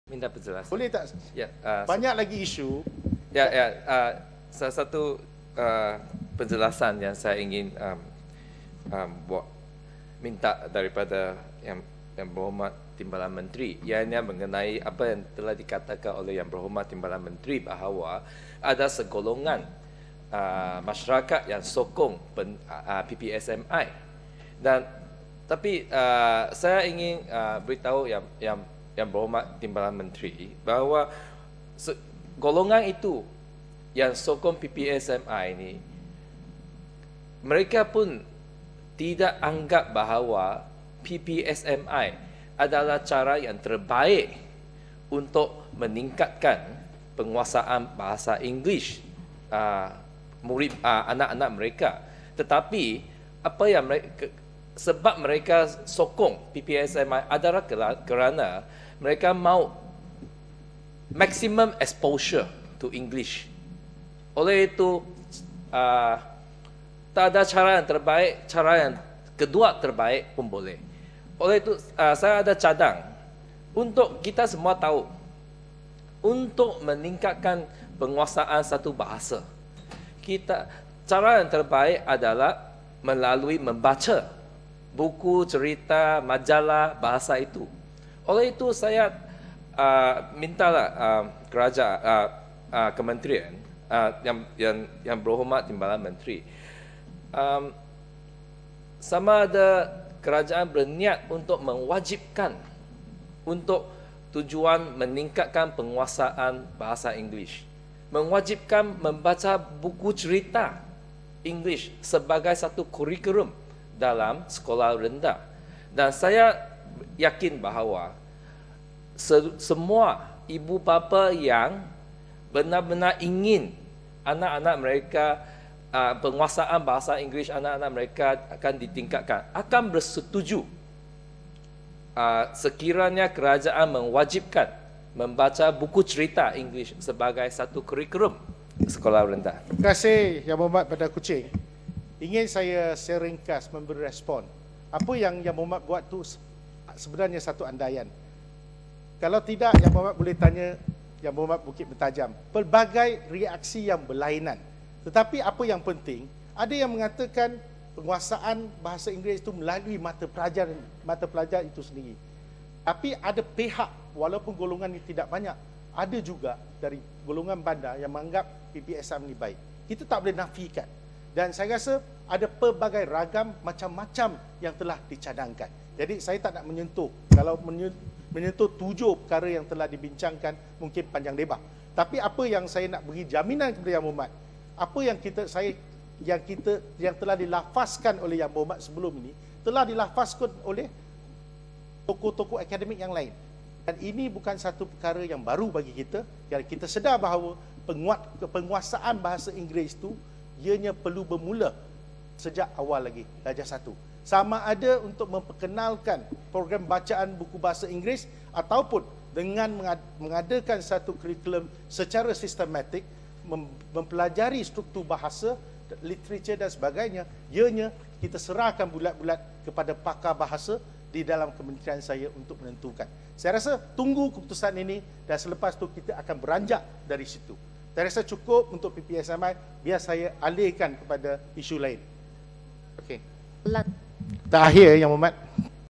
Chong Chieng Jen on PPSMI
Parliament of Malaysia
Motion of Thanks Debate - Ministerial Reply
2 March 2009